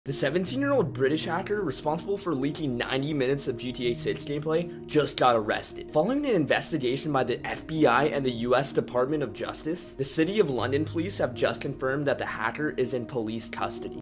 GTA 6 Hacker Arrested By The FBI
The GTA 6 hacker has been caught by the FBI and has been arrested by the London police.